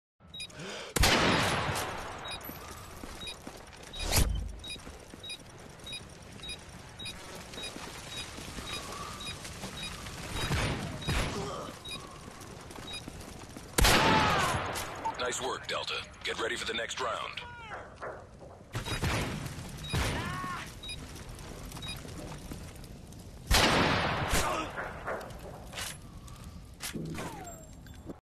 balurde - F*cking Style on Search & Destroy!
Sniper Game Clip